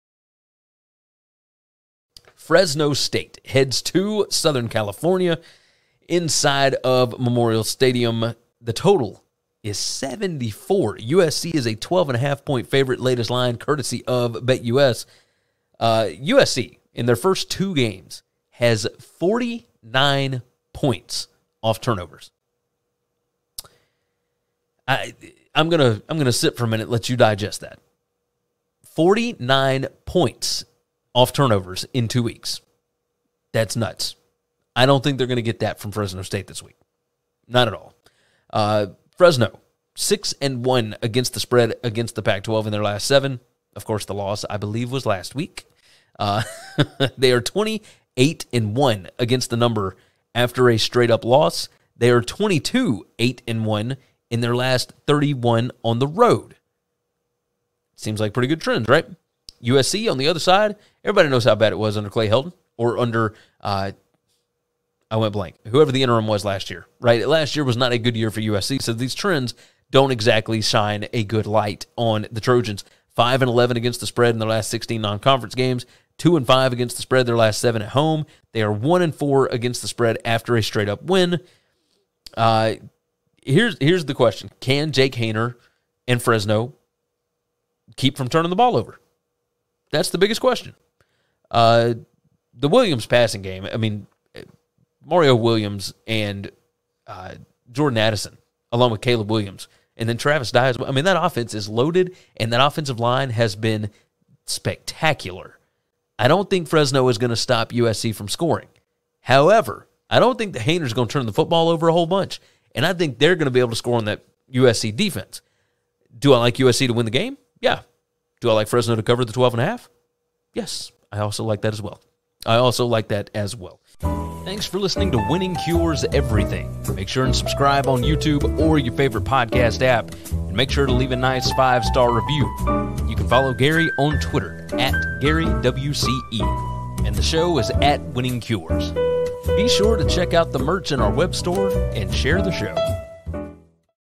Fresno State vs USC Picks Against the Spread 2022 College Football Predictions
Fresno State vs USC Picks Against the Spread 2022

Gary gives a quick preview, predictions & his pick against the spread (ATS) for the 2022 College Football Week 2 matchup featuring USC vs Fresno State!

To see Gary’s OFFICIAL spread picks & best bets, along with more in depth breakdown of more CFB games,check out this week’s BetUS College Football Show: